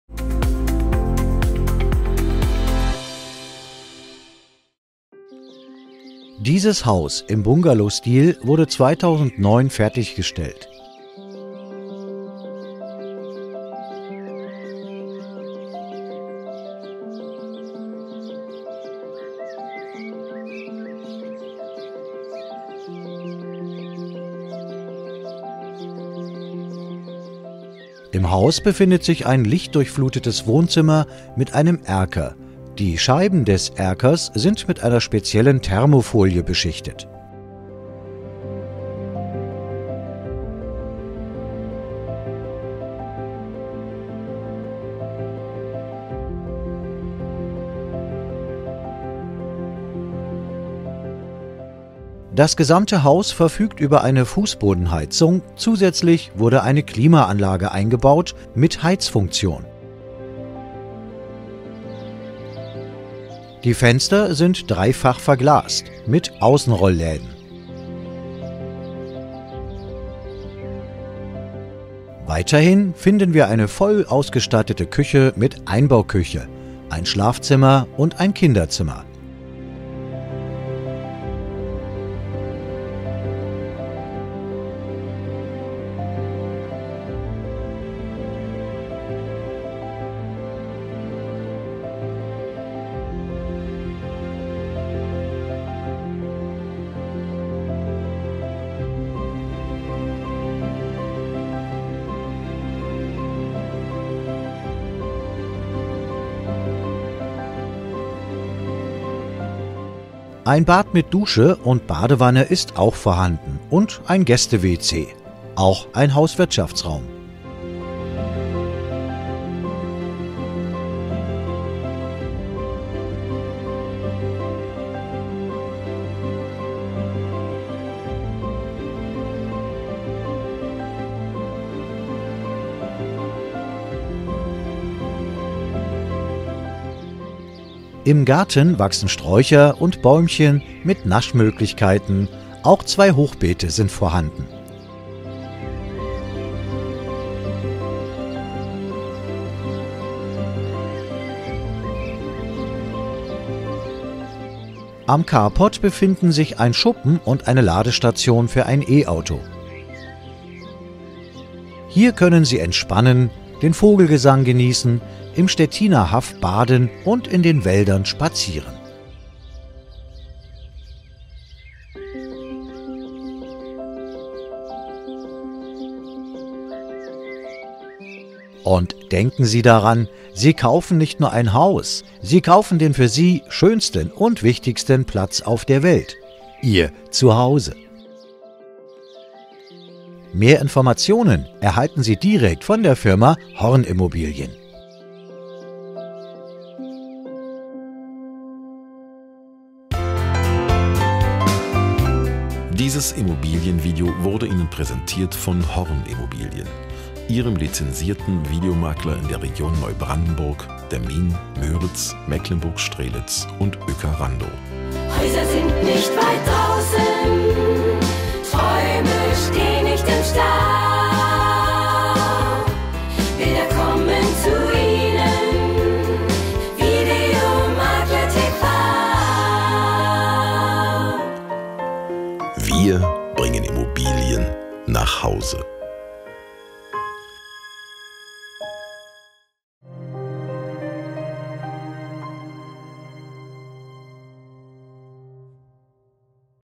HORN IMMOBILIEN ++ Einfamilienhaus im Bungalowstil bei Ueckermünde
Energieausweis (Bedarfsausweis)
118,30 kWh / (m²*a)
Endenergiebedarf
Weitere Informationen

Wesentlicher Energieträger Flüssiggas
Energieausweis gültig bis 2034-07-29
Energieausweis Jahrgang ab dem 1.5.2014
Energieausweis Werteklasse D
Energieausweis Baujahr 2009
Energieausweis Gebäudeart Wohngebäude
Heizung Zentralheizung
Befeuerung Gas